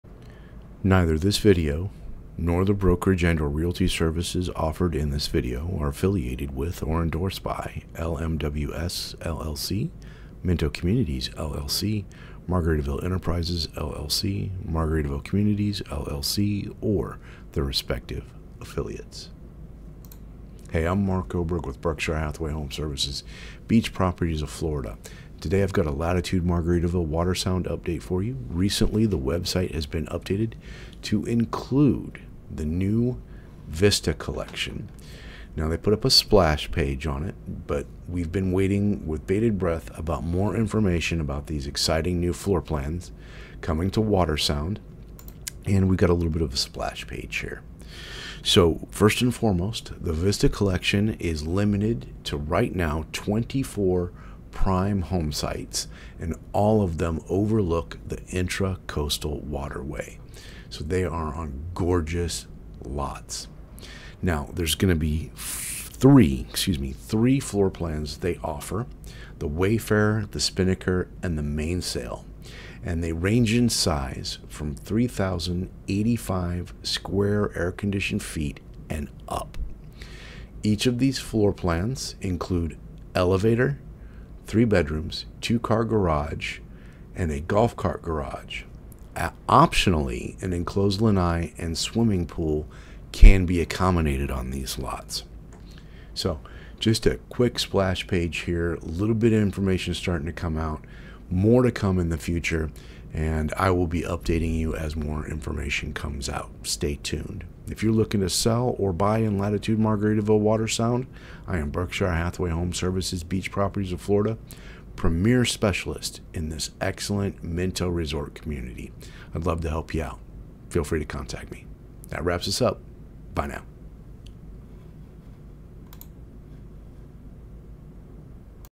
Latitude Margaritaville Watersound: Agent Update ~ Vista Collection
A few new details about the Vista Collection at Latitude Margaritaville Watersound.